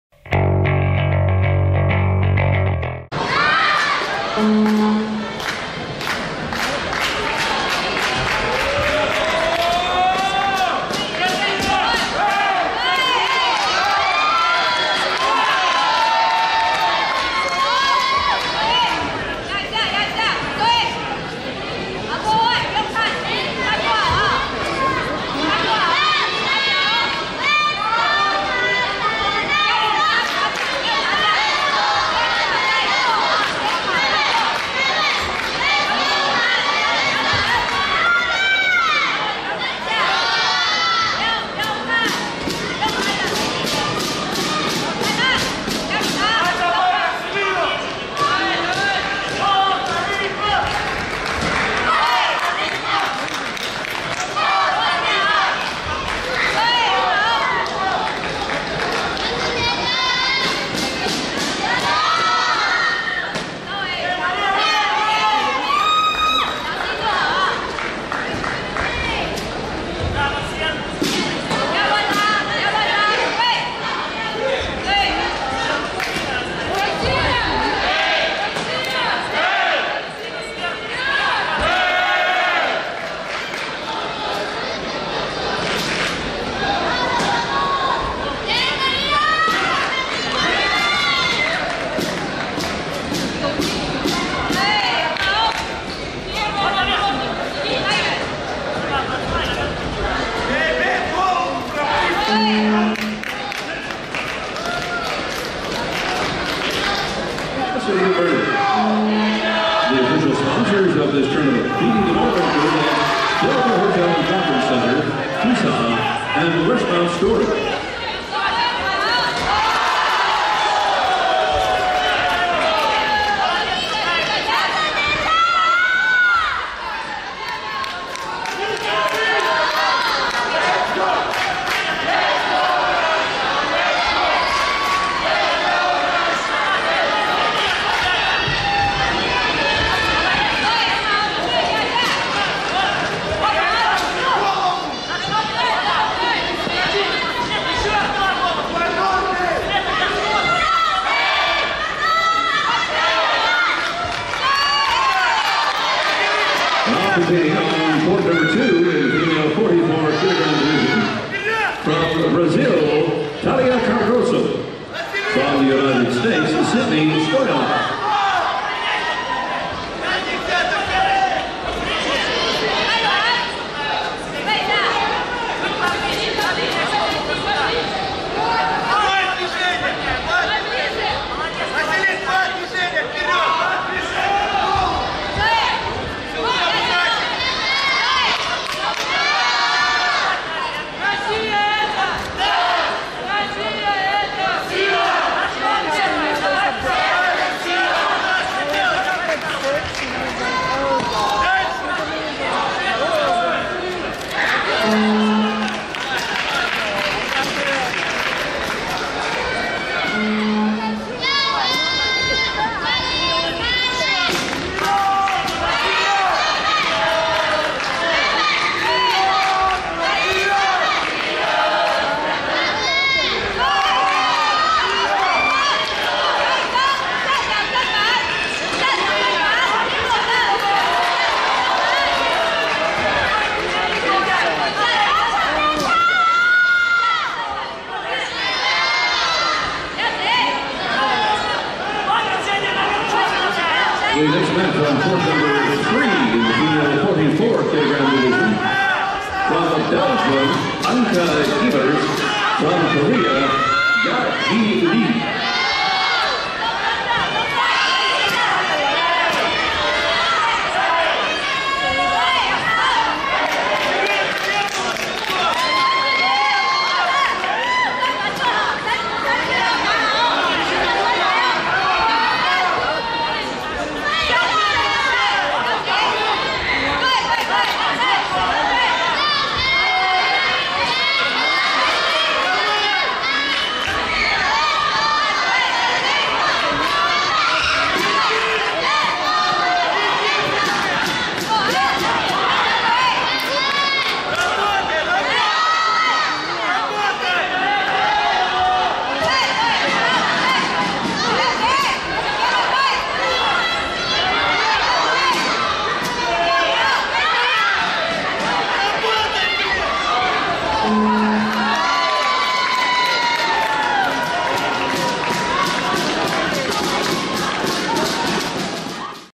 PODREZ Vasilina(RUS)-CHEN Jeih Die(TPE) Female 44kg (1/16)
16-20 November 2016 WTF World Taekwondo Junior Championships in Burnaby Canada